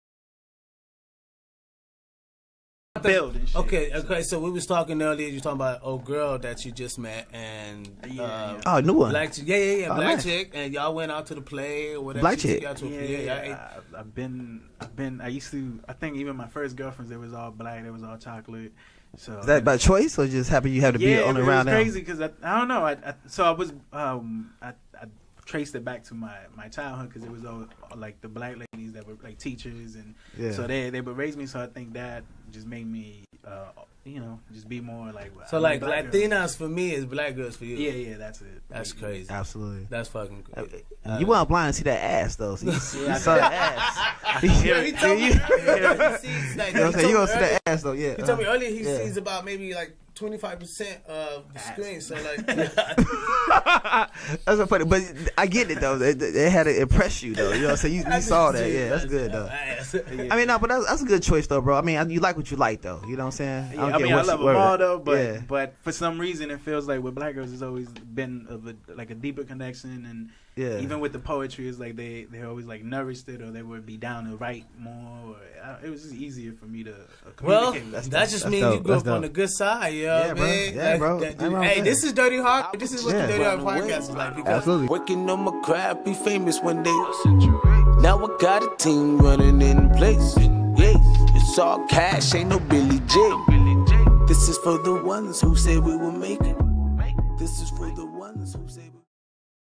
Gasmasktribe TV | Latinos who loves black women
@DirtyHarvard(music) podcast- hosted by lead vocalist/drummer BIZ' the entertainer.

Guvie Malito tell his story his love for black women also his motivation for spoken word and speaking life into to people who's in the same shoes.

Follow Guvie Malito:
Instagram.com/GuvieMalito